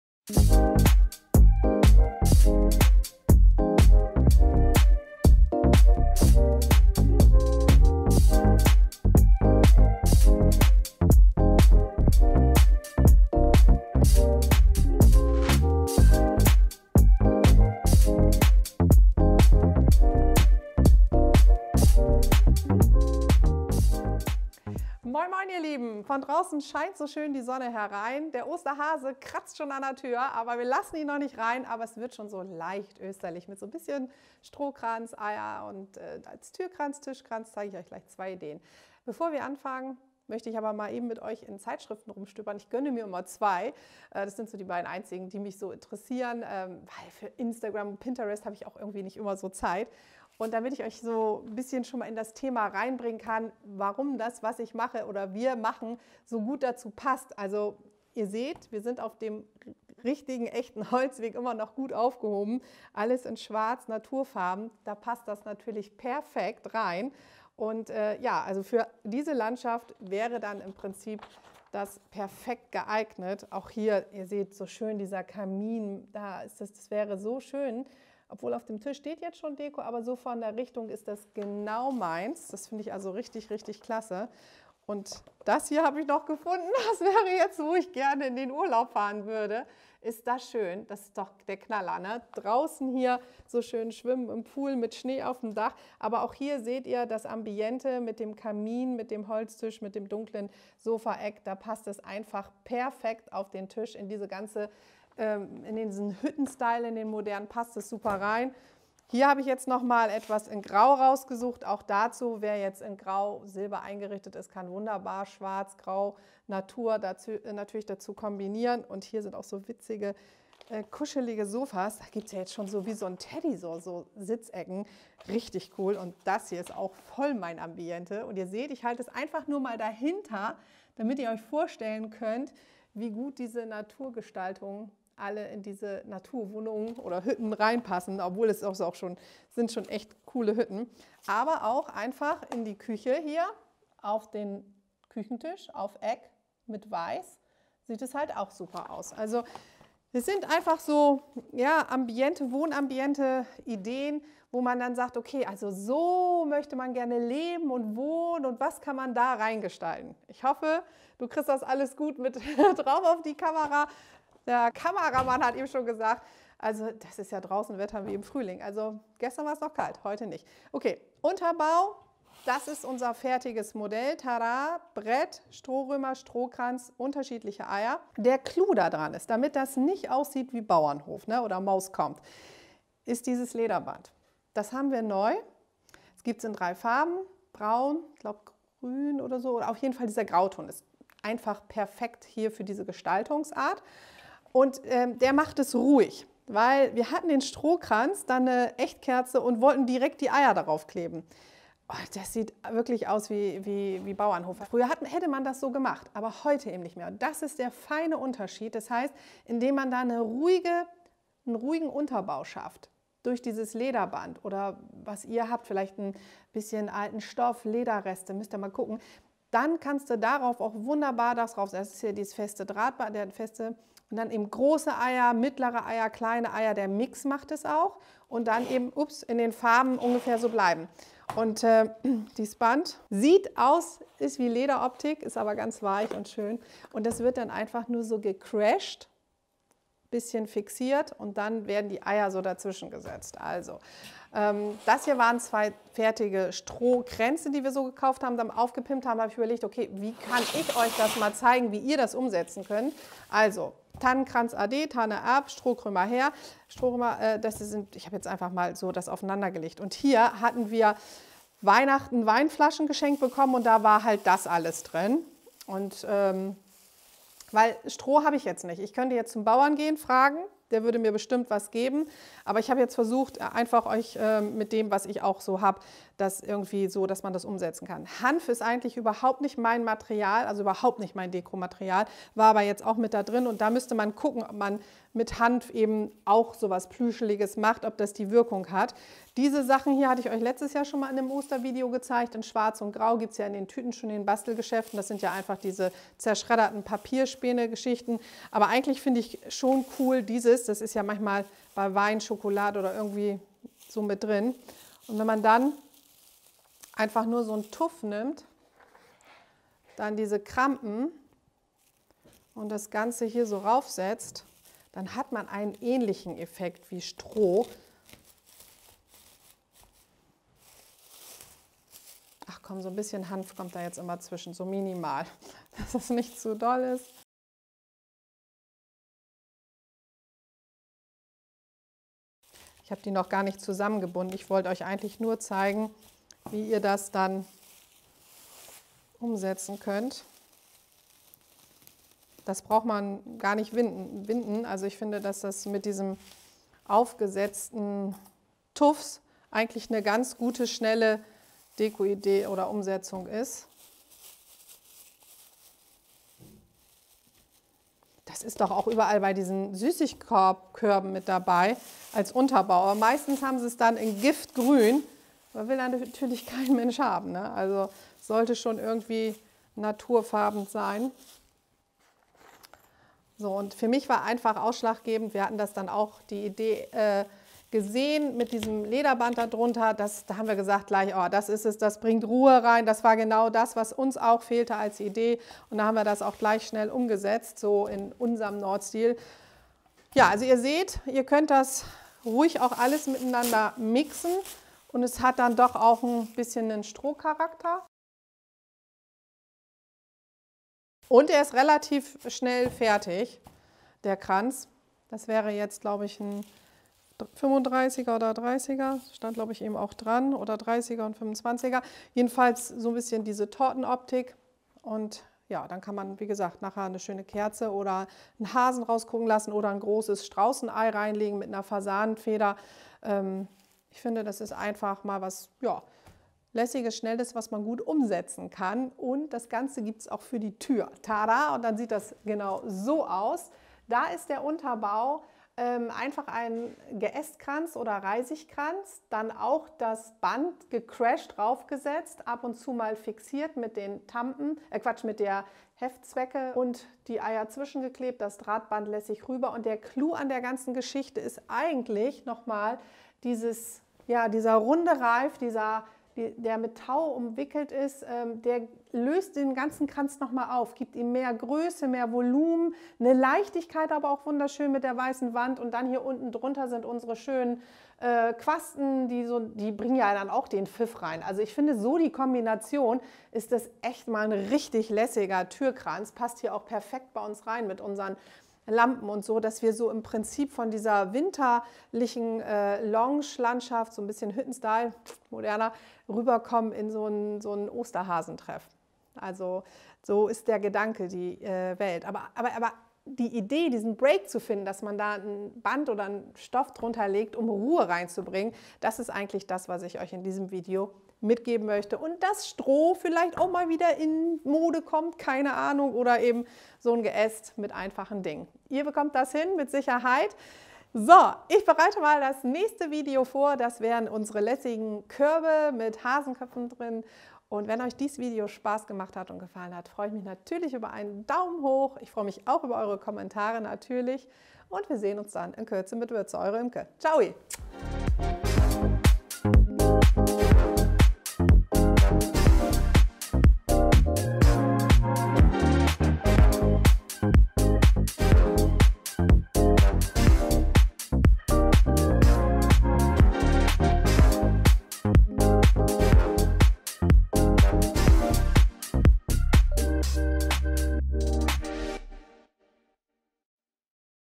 Moderner Ei-Kranz |Imke Riedebusch
Moin Moin Ihr Lieben,
wer hätte das gedacht, dass ein normaler Strohkranz so cool aussehen kann? Sicher liebt ihr genau diese Art von Gestaltung, einfach, Material an welches man ran kommt und der Preis stimmt auch noch. Perfekt, also gleich ran an die Umsetzung. Ich wünsche euch viel Spaß dabei,
Eure Imke

Video Gerjet Nordhausen